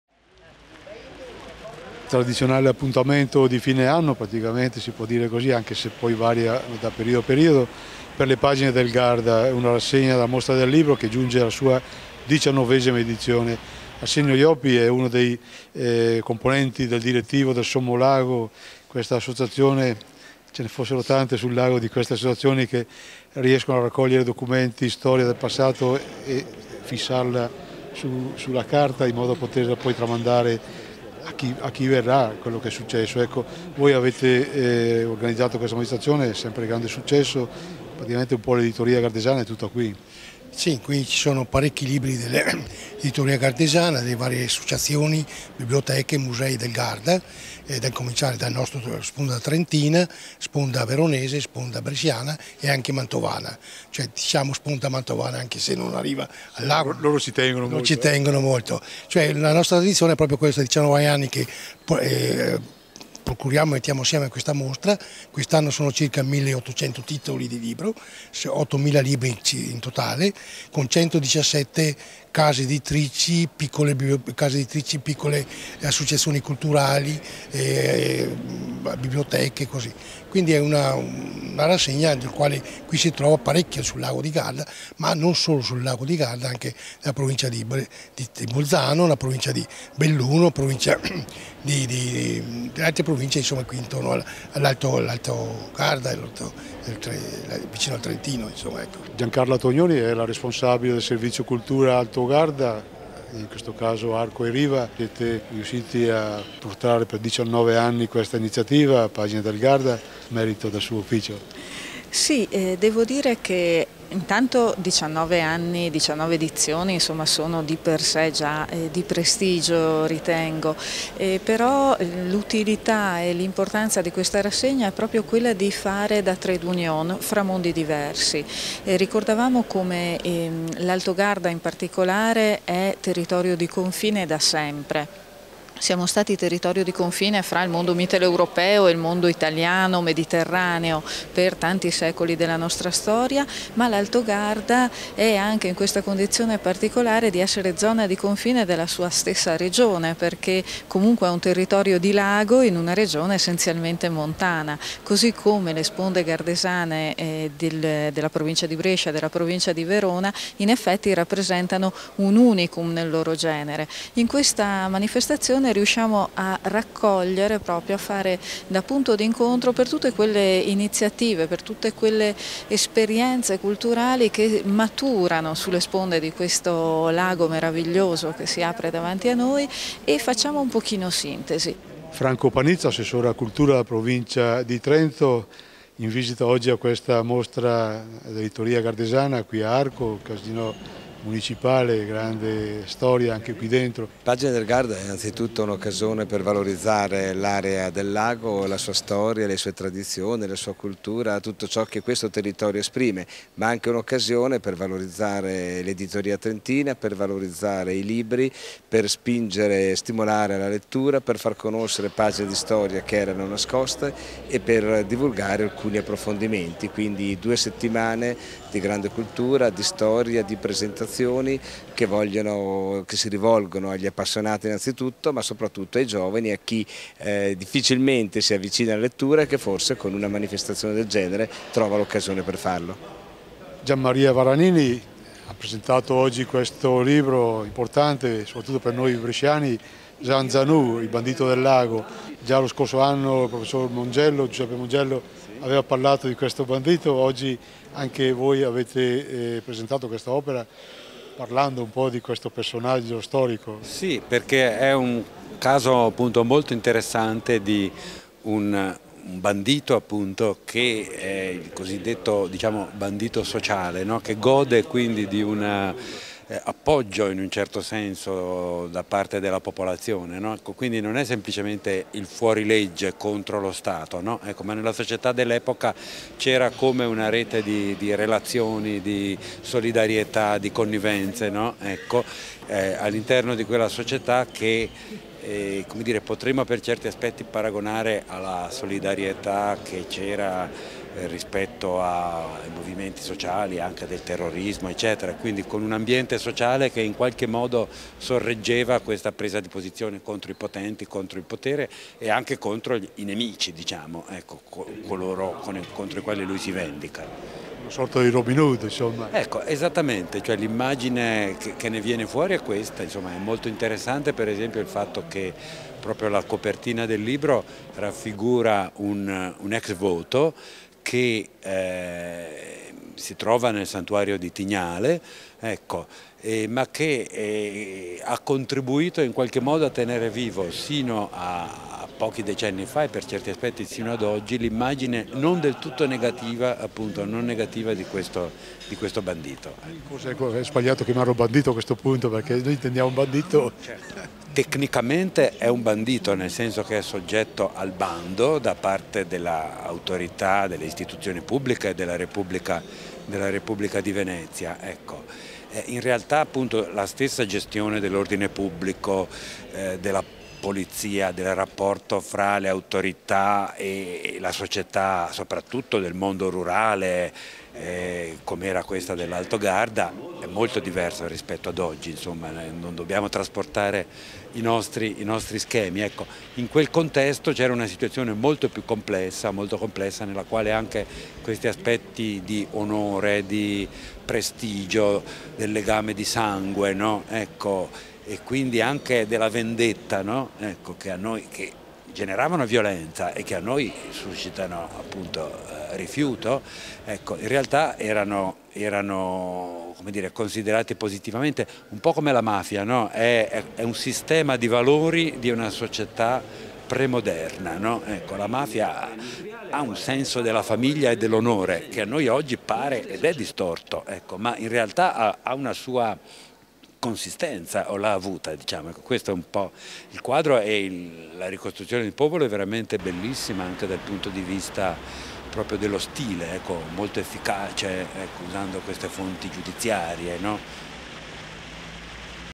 Pagine del Garda
Anche quest'anno il Casinò di Arco di Trento ha ospitato "Pagine del Garda" la rassegna dell'editoria Gardesana e, come tutti gli anni, un convegno a fatto da corollario all'evento, il teme di quest'anno è stato il bandito Zanzanù, una figura che negli ultimi anni sta godendo di una rinnovata popolarità.